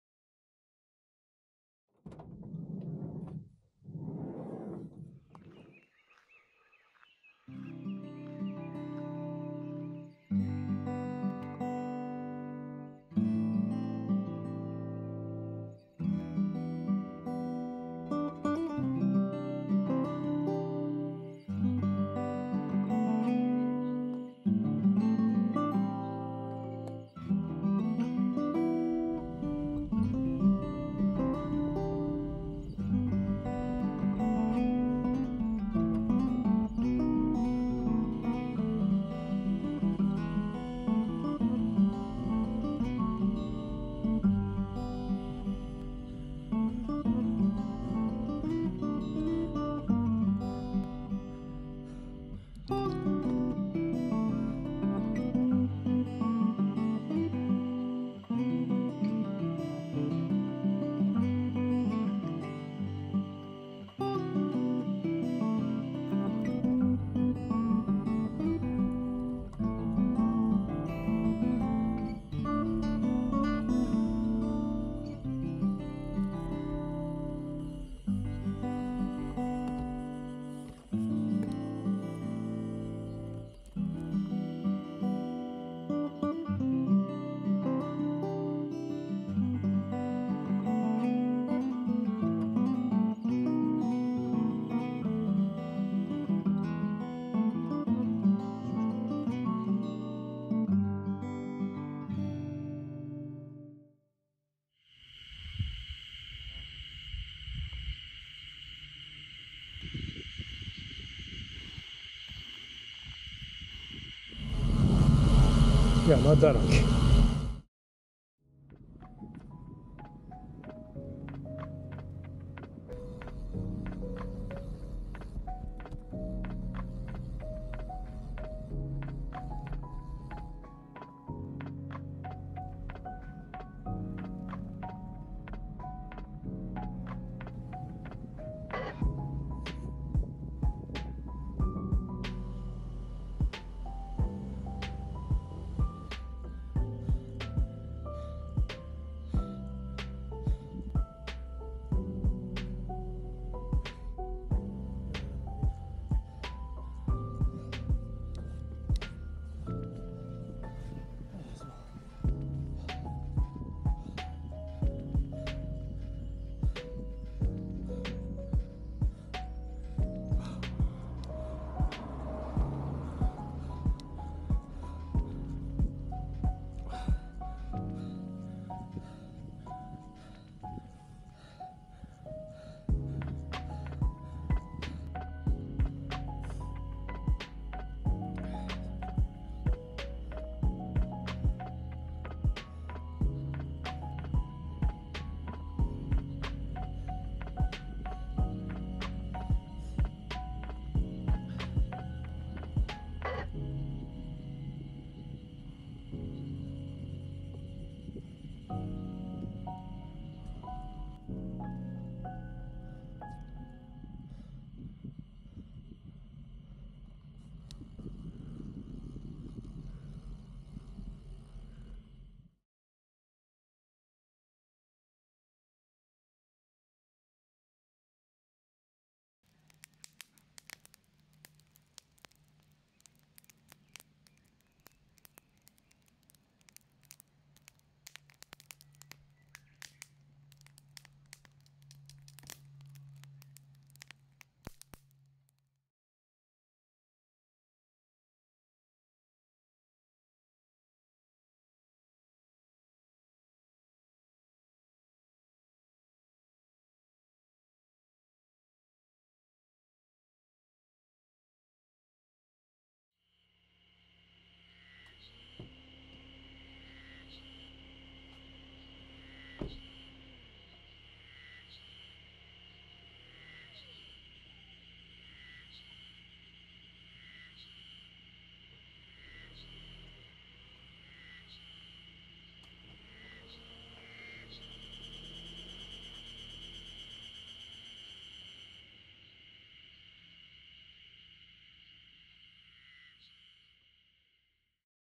【キャンプ✖︎ロードバイク】６輪旅✖︎ゆるポタ＃10 車中泊キャンプと茶臼山ヒルクライム
二泊三日で長野県のほぼ最南端「南信州　売木村」の星の森オートキャンプ場で車中泊をしながら、県境の山、茶臼山ヒルクライムを楽しんできました。
天気もよく、梅雨の合間の晴れにうまくハマったので、とても楽しいキャンプライドでした。
今回、うまくできたので、今後は車中泊キャンプをしながらの旅も問題なく行けそうです。
楽しみが広がります。